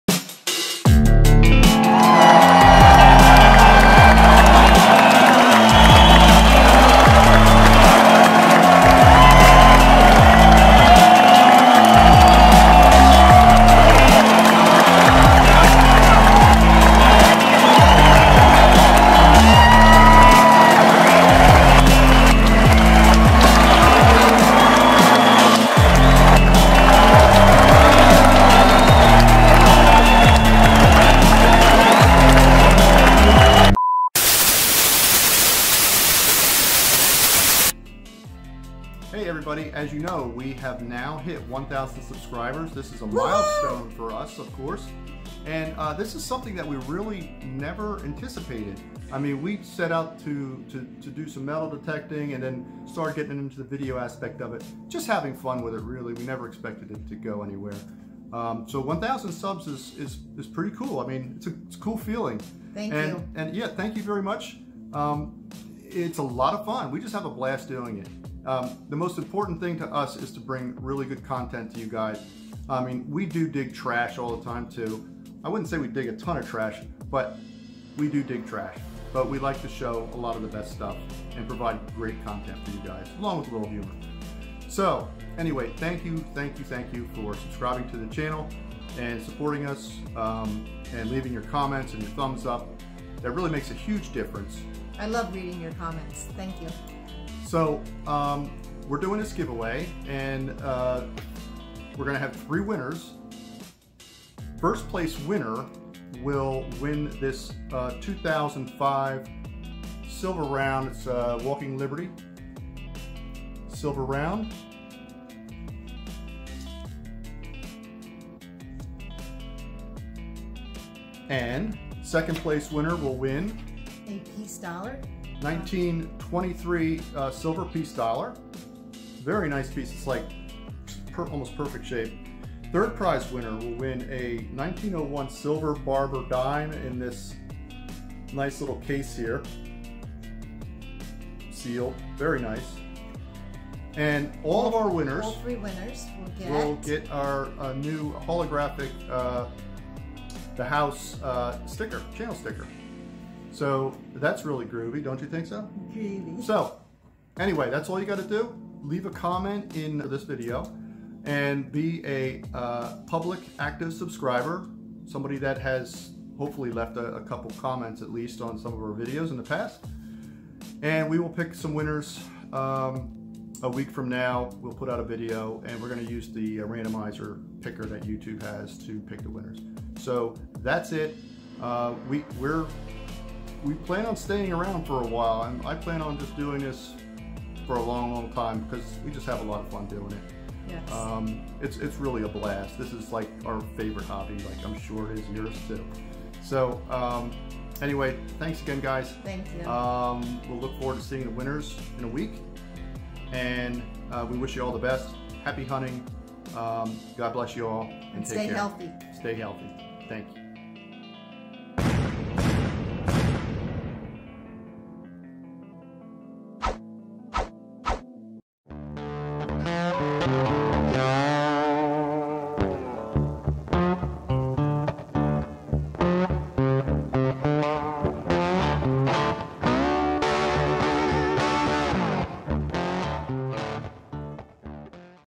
1000 Subscriber GAW! Metal Detecting
We are giving away 3 nice items as a thank you for supporting our channel over the past year and reaching 1000 subscribers!  In order to be eligible you must be a public subscriber.  We will be posting a video next week around August 3rd announcing the winners.  THANK YOU All and we hope we can continue to be friends !!!